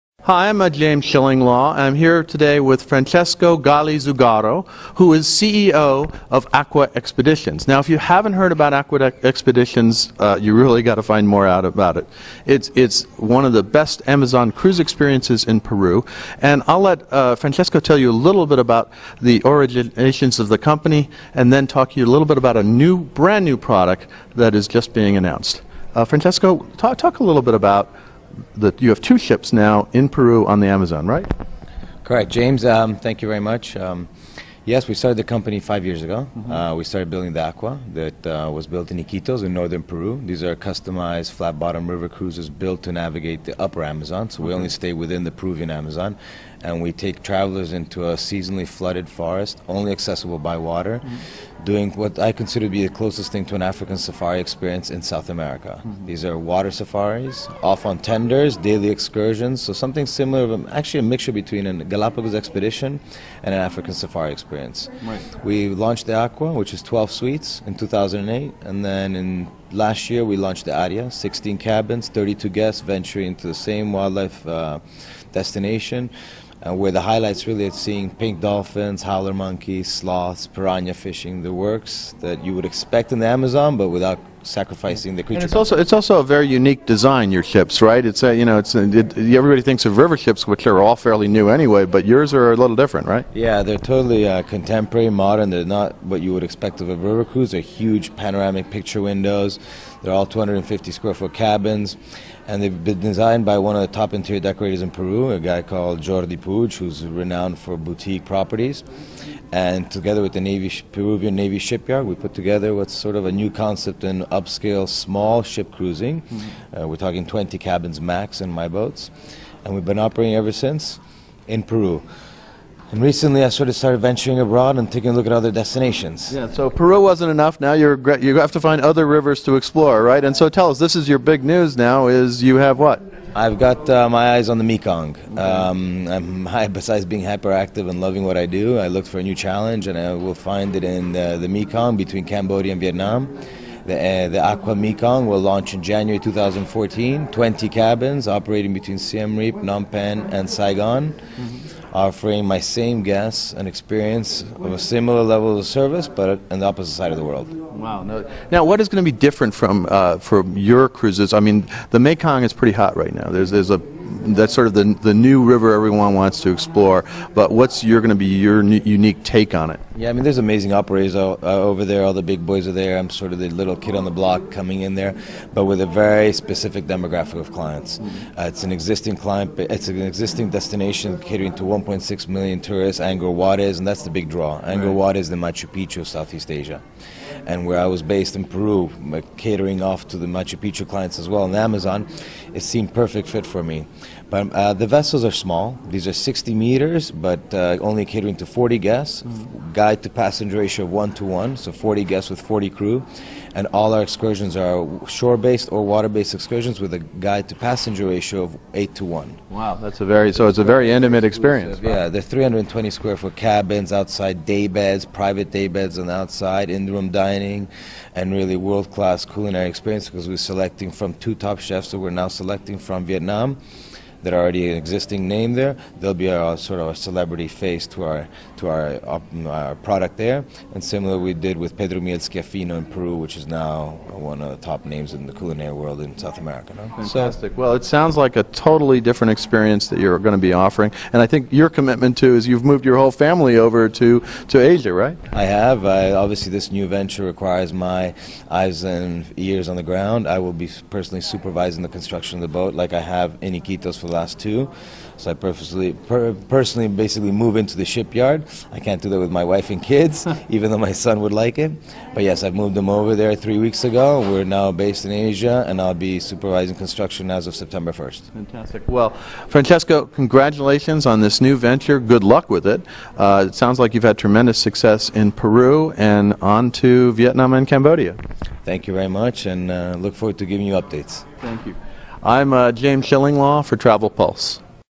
Aqua Expeditions Launches Luxury Cruises on the Mekong River in 2014
Aqua Expedition's Founder and CEO, Francesco Galli Zugaro is interviewed by James Shillinglaw from TravelPulse about Aqua Expedition's expansion into Vietnam and Cambodia, offering luxury cruises on the Mekong River beginning in early 2014.

Join our Luxury cruises through the Mekong River.